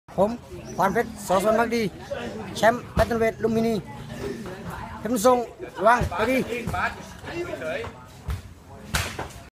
KICK Insist5 kwanpet sor.swanpakdee
新日本キックボクシング協会 KICK Insist5 11・15ディファ有明 メインイベント ムエタイスペシャルマッチ 56K契約3分5R クワンペット・ソースワンパクデー（泰国ルンピニースタジアム認定バンタム級王者／タイ）vs ペップンソーン・ペッチンダー（CPガイトーン ガンラーントーナメントバンタム級王者・プロムエタイ協会バンタム級9位／タイ）